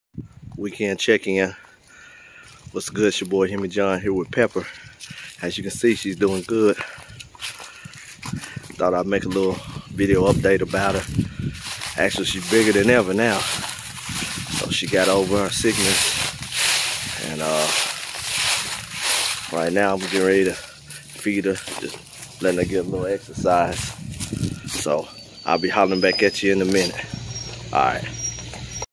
Pepper my 180 lb female Mastiff
Quick video of my female English Mastiff pepper after she recovered from parvo